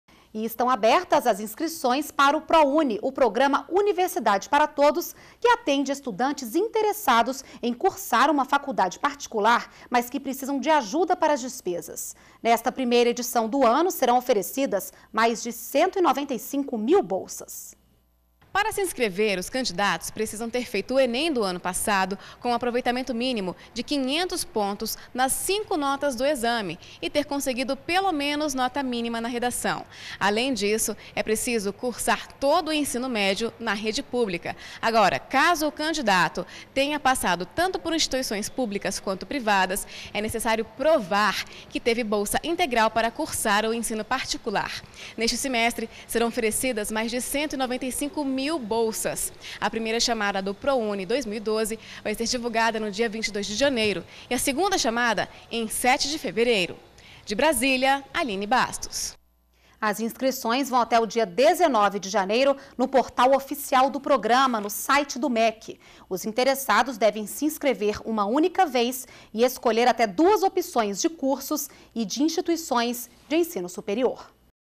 ProUni abre inscrições para mais de 195 mil bolsas em faculdades particulares
NBR Notícias - 14.01.12: Estão abertas as inscrições para o ProUni (Programa Universidade para Todos), que atende estudantes interessados em cursar uma faculdade particular, mas que precisam de ajuda para as despesas. Nesta primeira edição do ano, serão oferecidas mais de 195 mil bolsas. As inscrições vão até o dia 19 de janeiro no portal oficial do programa, no site do MEC. Os interessados devem se inscrever uma única vez e escolher até duas opções de cursos e de instituições de ensino superior.